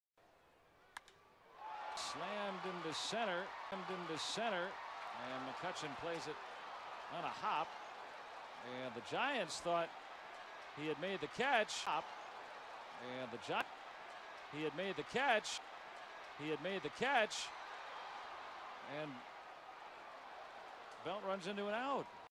Andrew McCutchen cuts down belt with perfect throw
Andrew McCutchen dives and traps Buster Posey's single, but gets up and rifles a throw to third base to nail Brandon Belt